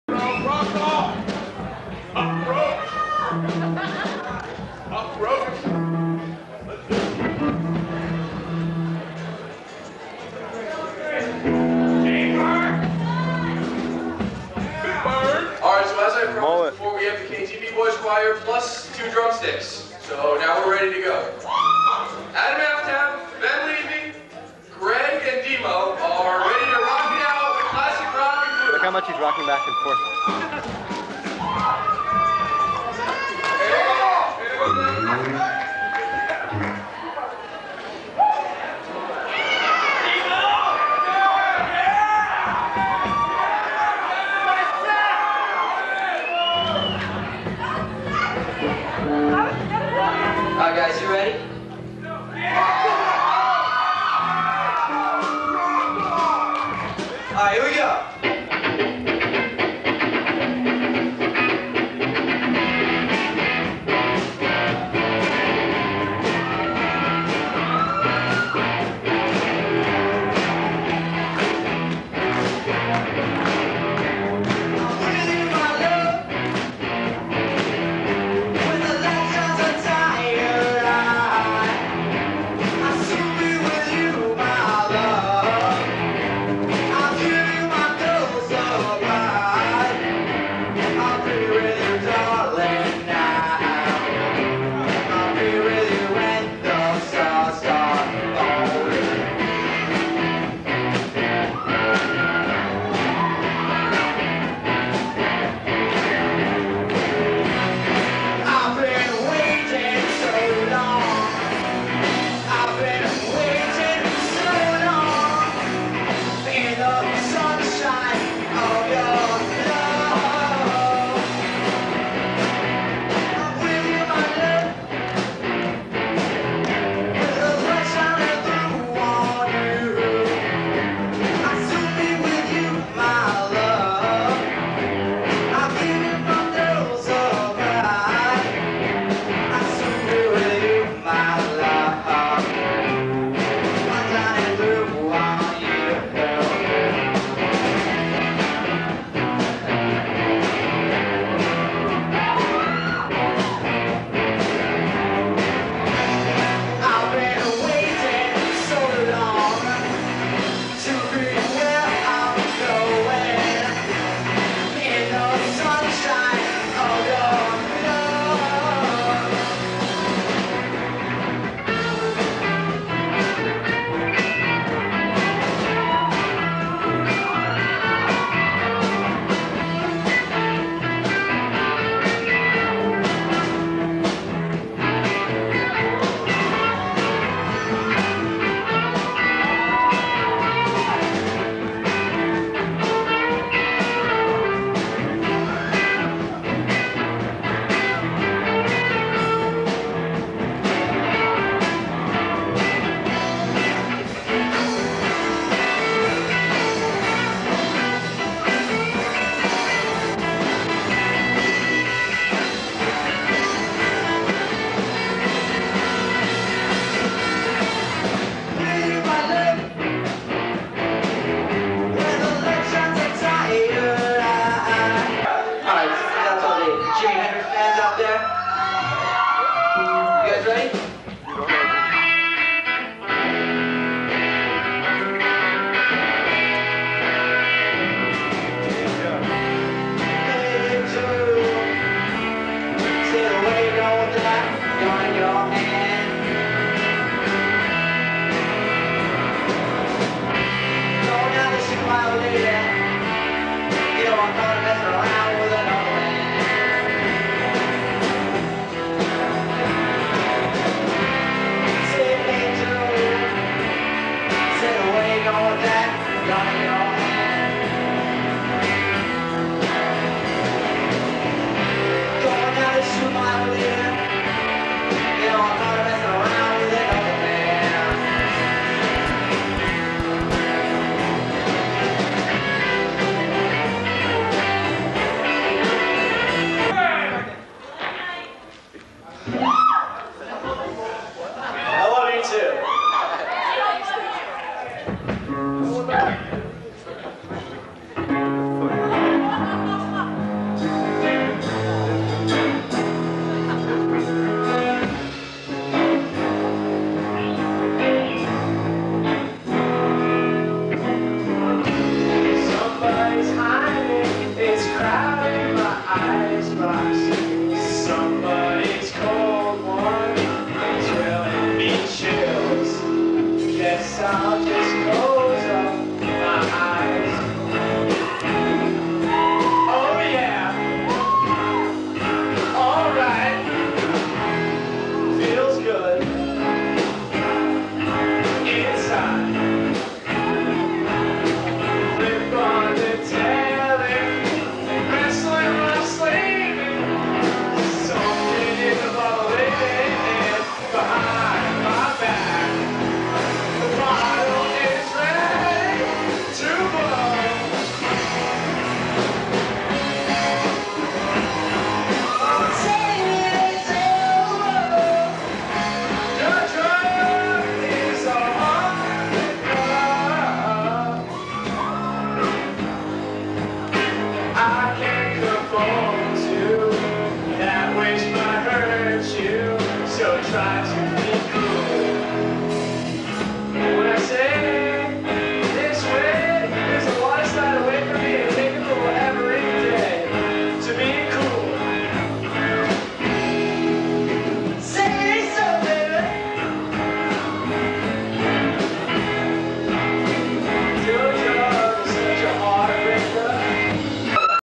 Spanish River Show '01
KGB Boys Choir Featuring music by Weezer, Vampire Weekend, Adam Aftab, Ben Levy, Demo, Greg Schecterman, Mike Volz, and Rob Slavin, Jimi, and Cream